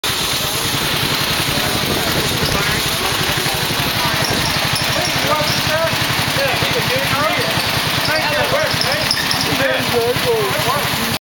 Drivable Alligator
This drivable alligator also breathes fire.